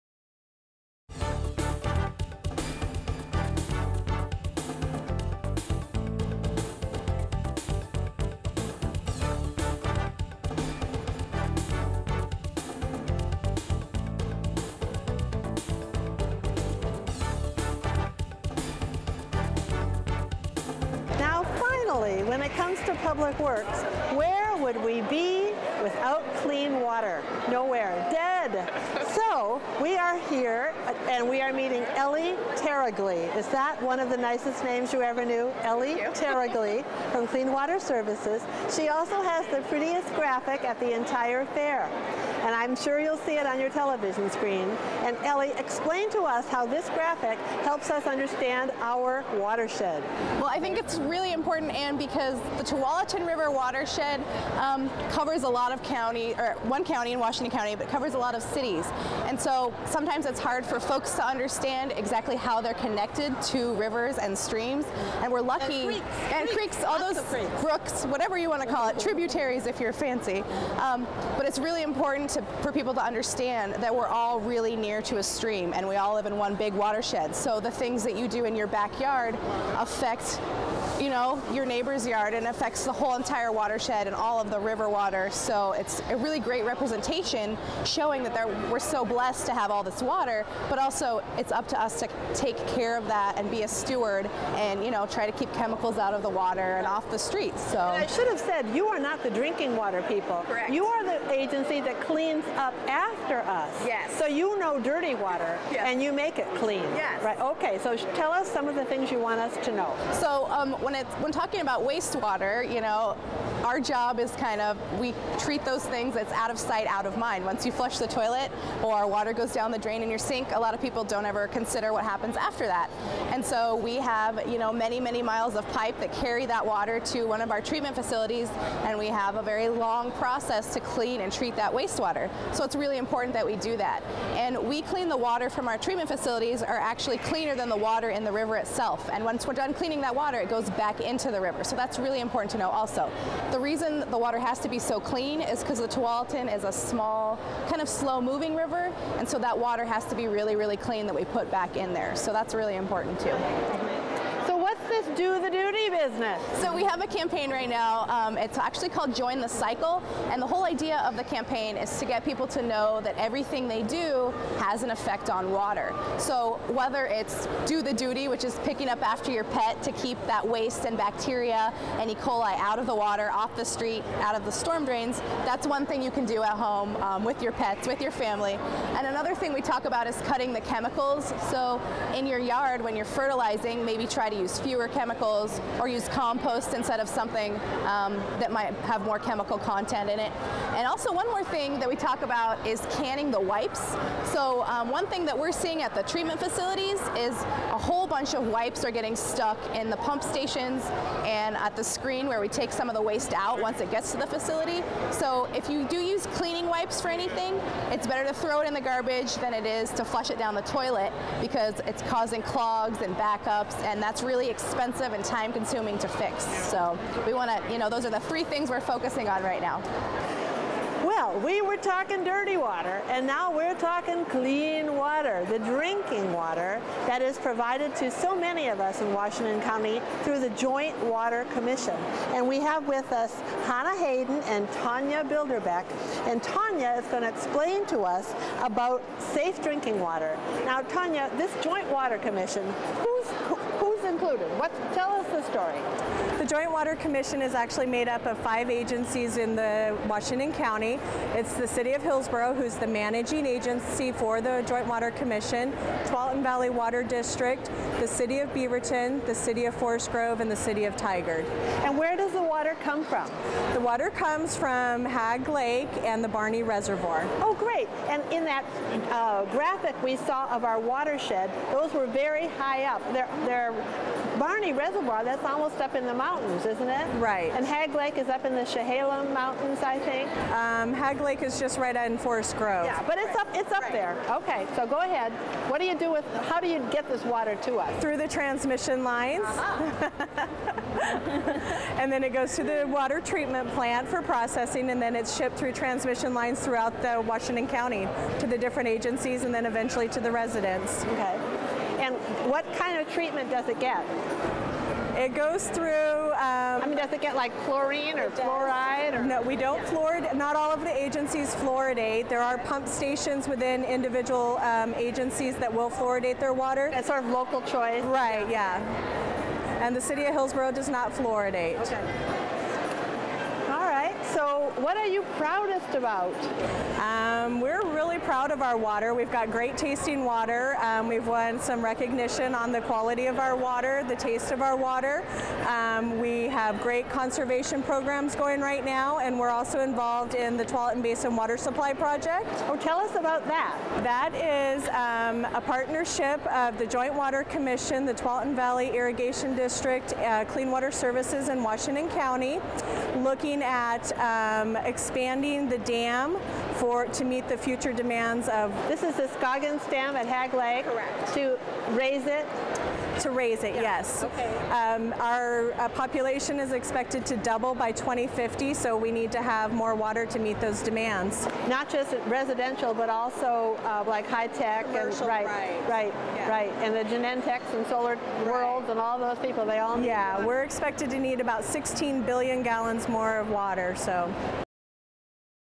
Washington County - National Public Works Week - Water
This segment focuses on the delivery and maintenance systems to provide water to the communities in Washington County